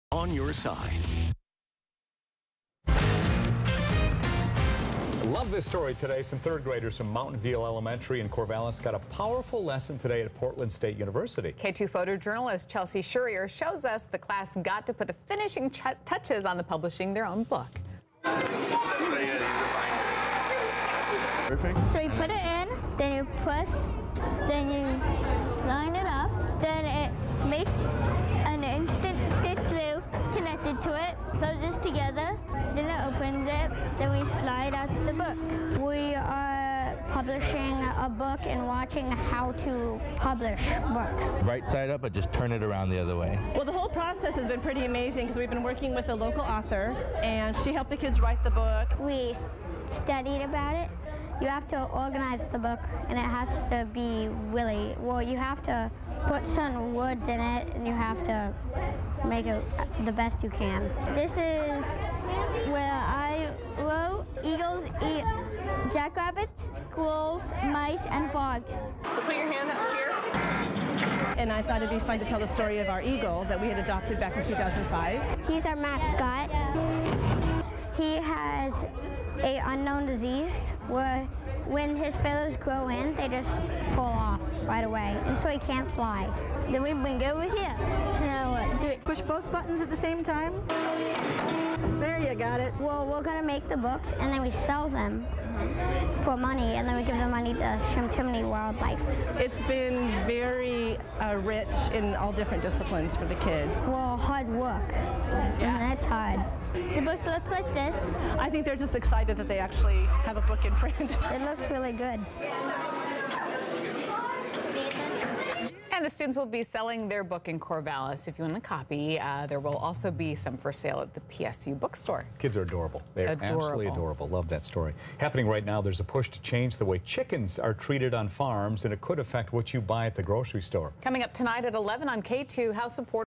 Sparky the Eagle Odin Ink Coverage Channel 2
Coverage of the Mountain View Elementary School visit to Portland State Bookstore to publish their Sparky the Eagle book.